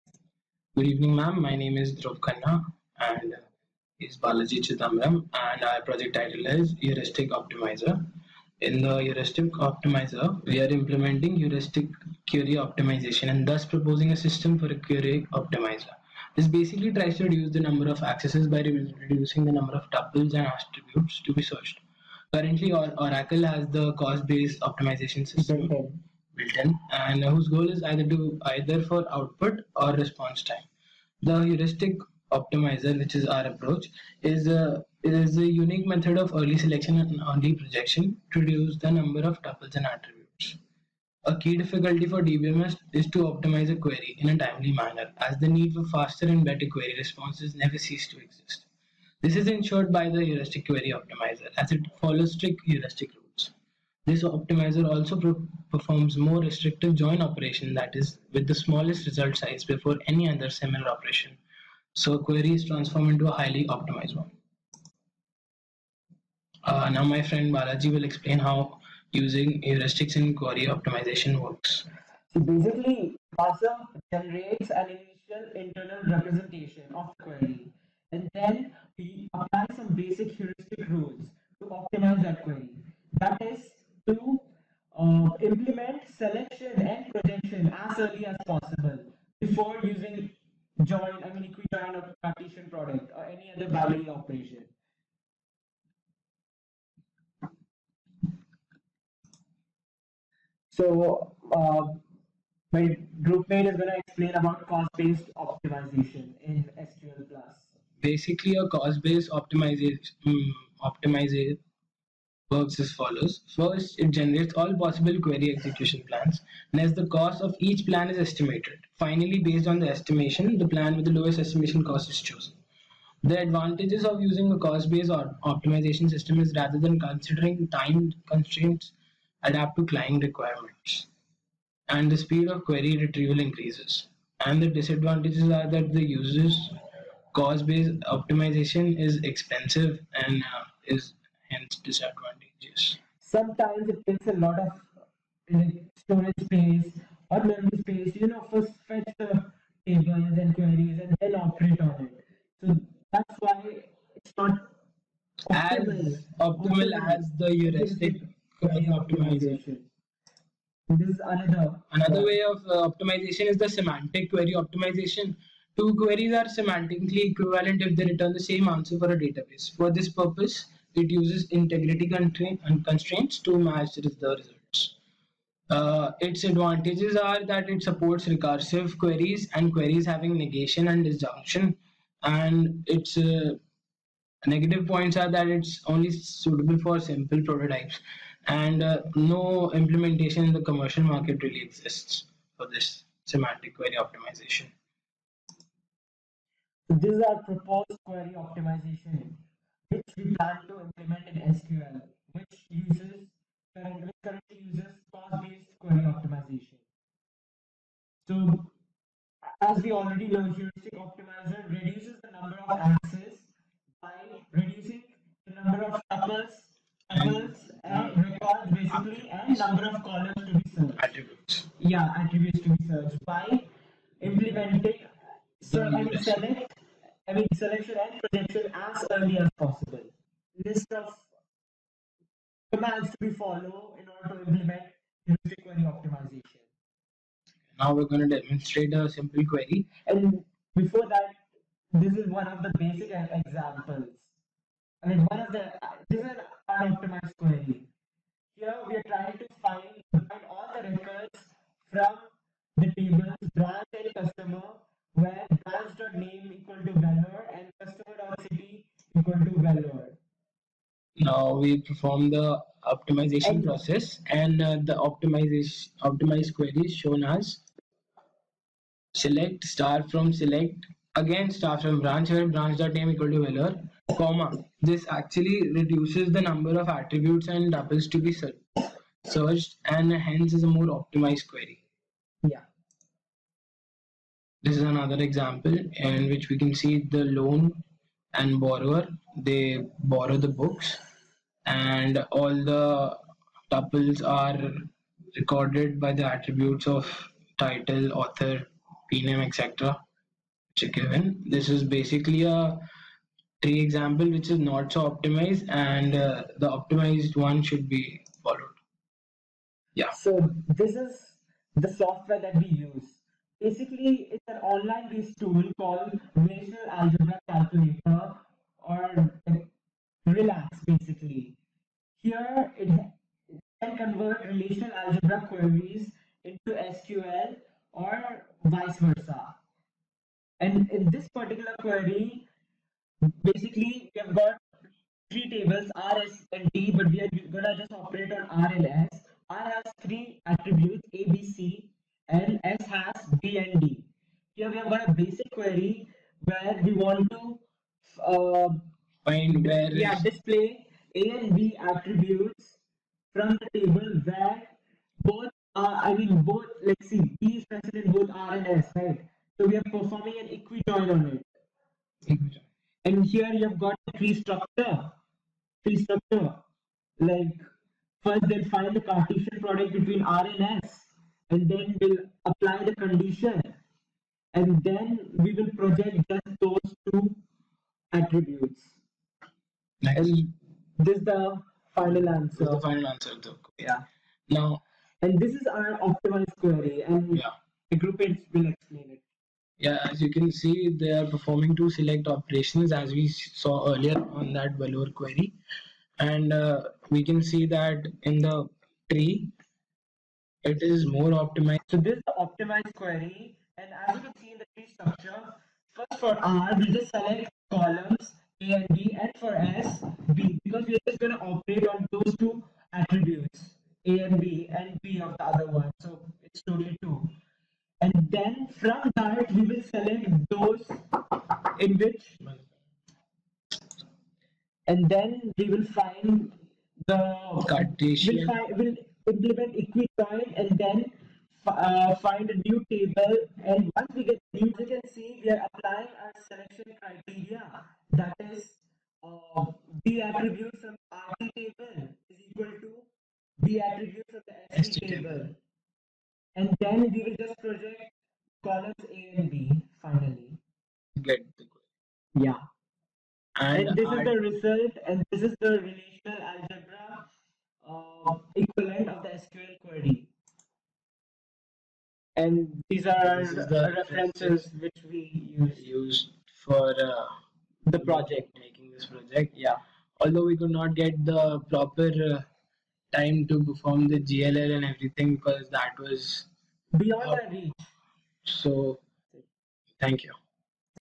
Heuristic Query Optimization in SQL DBMS Project
Database Management System Project
By
Balaji Chidambaram 15BEC0267
Dhruv Khanna 15BEC0409
D2 Slot
VIT University